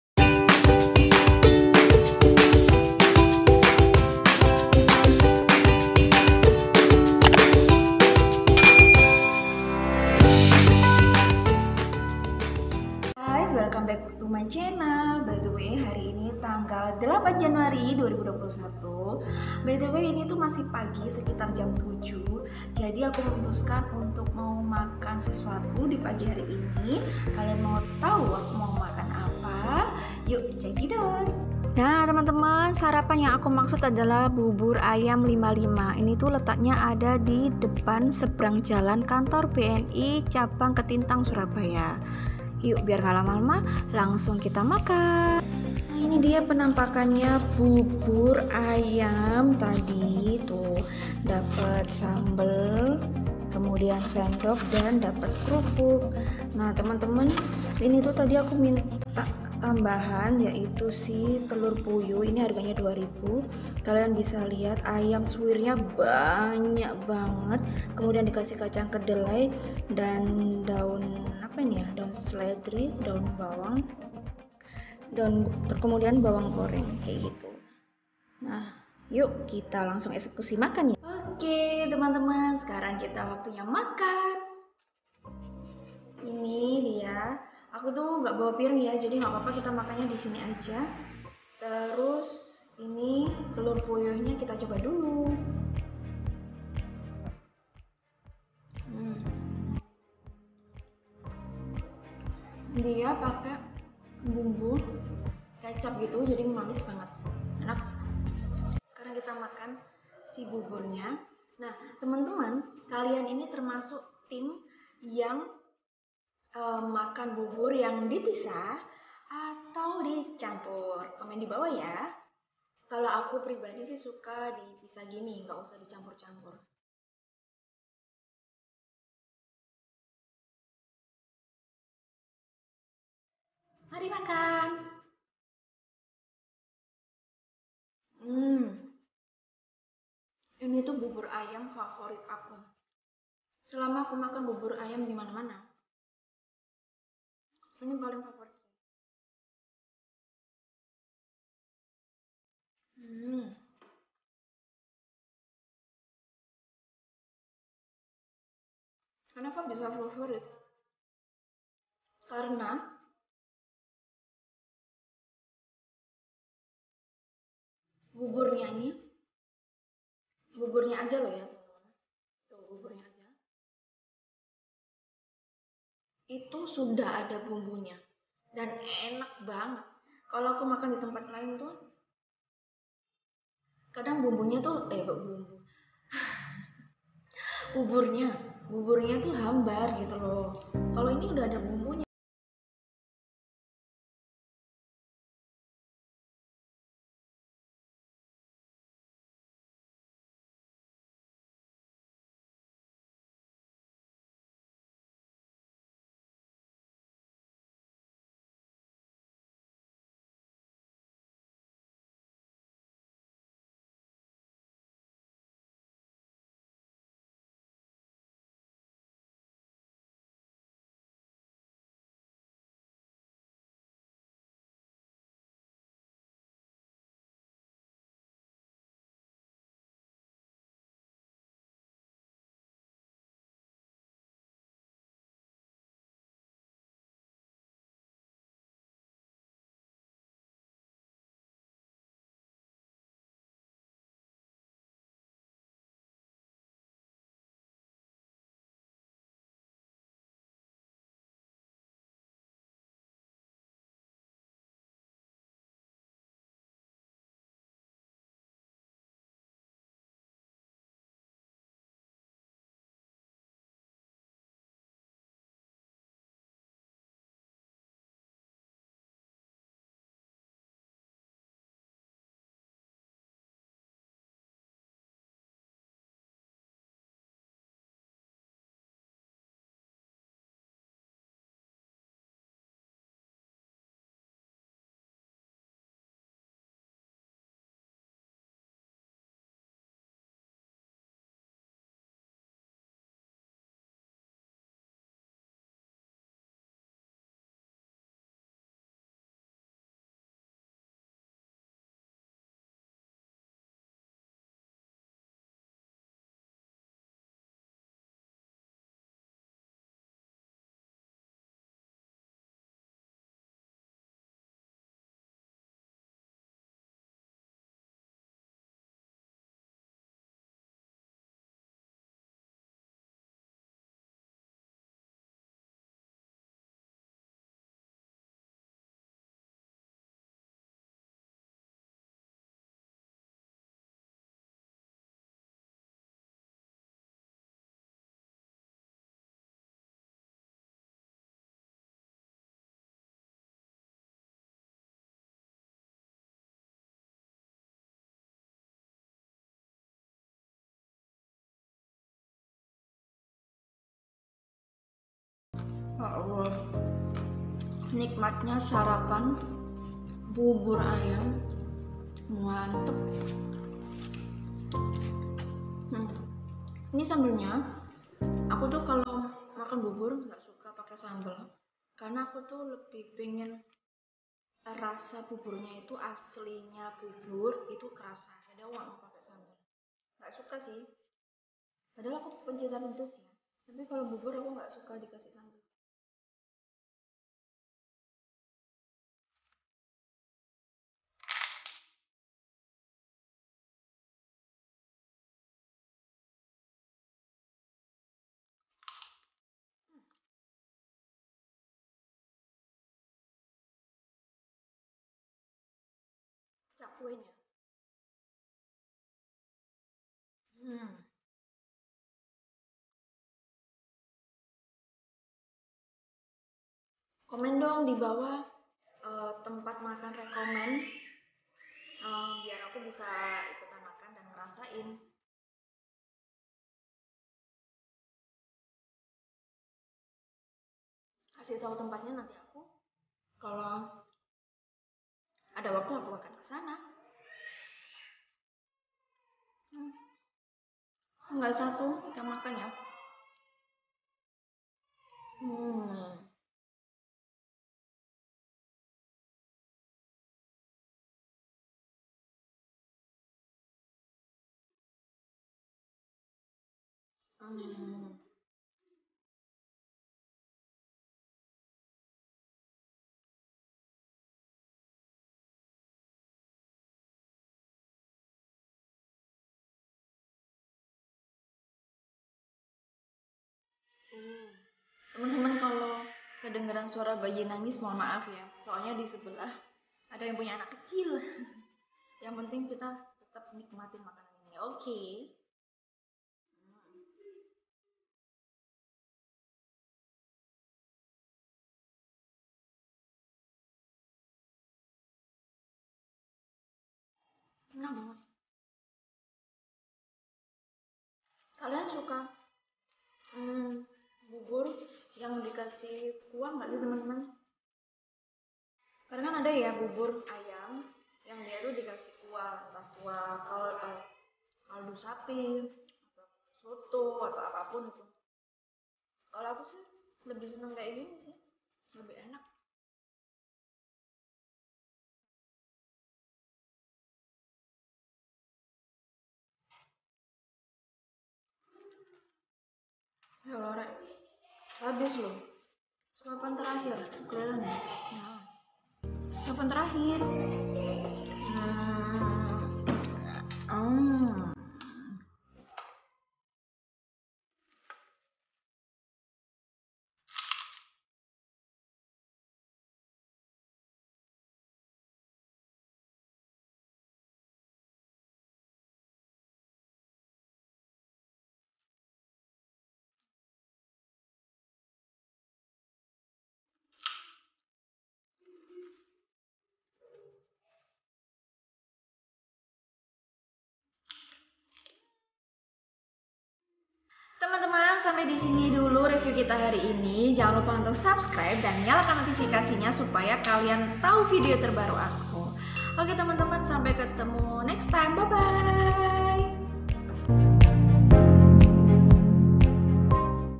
Terenak bubur ayam yang selalu laris manis !!!
Info Makanan:
Bubur Ayam 55
Alamat : Ketintang, Surabaya (Sebrang Jalan Bank BNI Cabang Ketintang Surabaya)